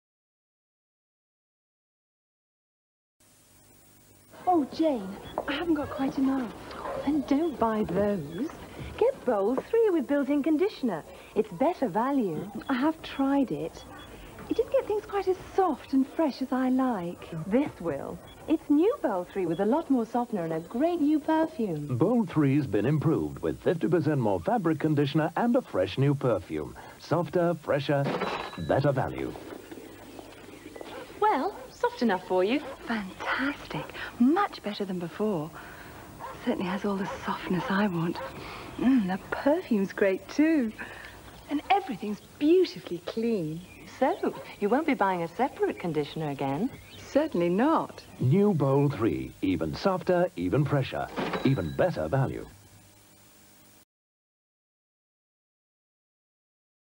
1984: Bold 3 Washing Powder [Jane]
Title: Softer
Brand: Bold
Advertiser/Client: Procter & Gamble
Media: TV Advert
Featuring: --
Year: 1985
Country: UK
Washing Machine: --
Model: --
Slogan: --